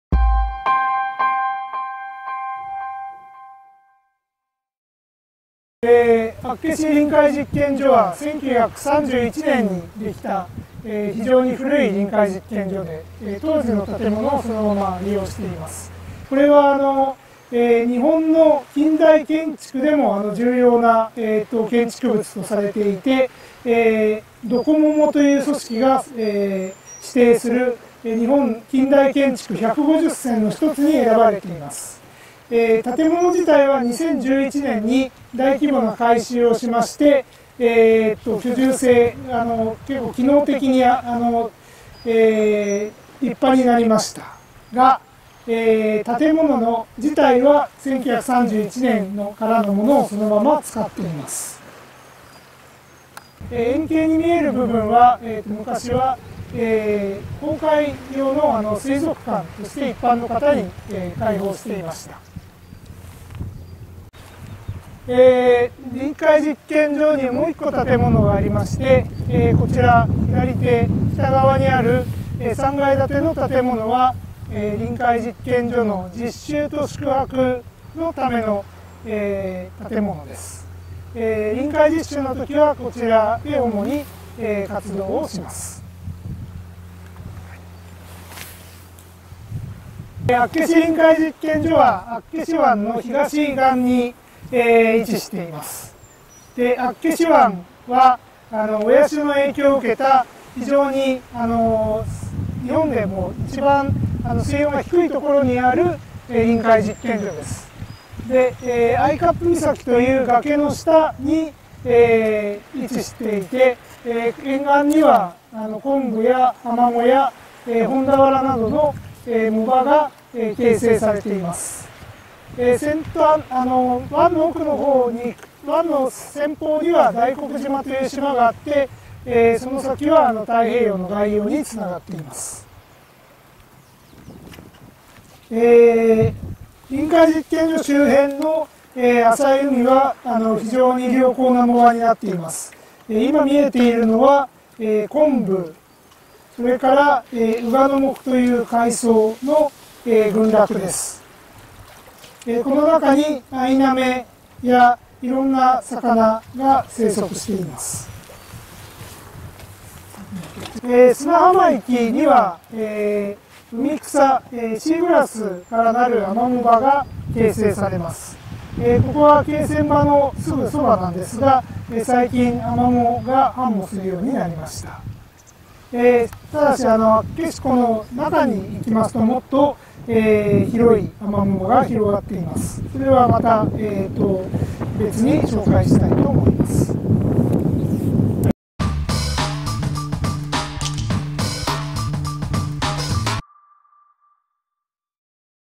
(Eng sub) 厚岸臨海実験所：実験所の紹介 (Akkeshi Marine Station: Introduction of the Laboratory)
Introduction of the appearance and surroundings of Akkeshi Marine Station,  Field Science Center for Northern Biosphere, Hokkaido University.

北海道大学・北方生物圏フィールド科学センター・厚岸臨海実験所の外観および周辺の紹介です。

動画撮影：安孝珍　（北海道大学・バランスドオーシャン運用部）
　　　　　長谷川貴章　（北海道大学・厚岸臨海実験所）
　　　　　田原聖　（北海道大学・厚岸臨海実験所）
動画編集：安孝珍　（北海道大学・バランスドオーシャン運用部）